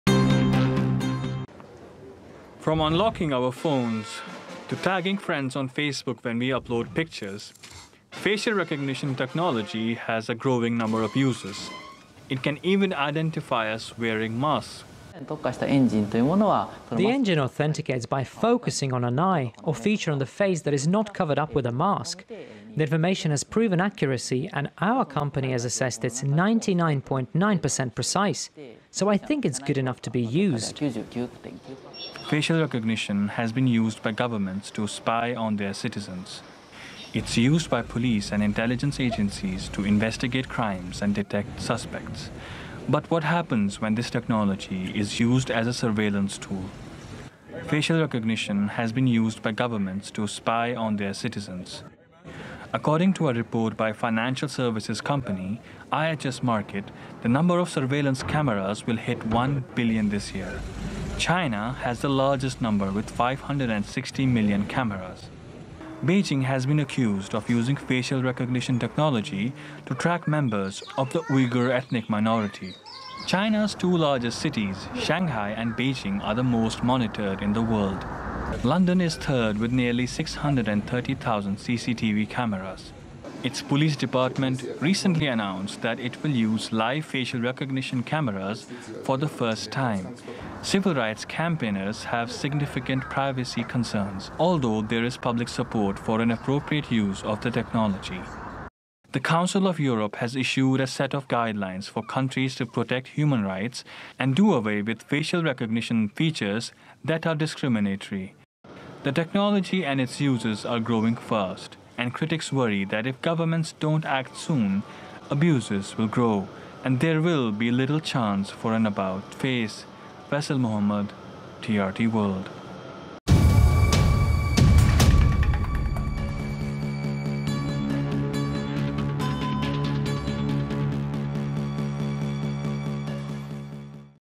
Report: Number of surveillance cameras to reach 1B in 2021
As facial recognition technology becomes an intrinsic part of our daily lives, the number of surveillance cameras across the world will reach 1 billion by 2021, with more than half of them reportedly in China. Faisal Mohammad reports.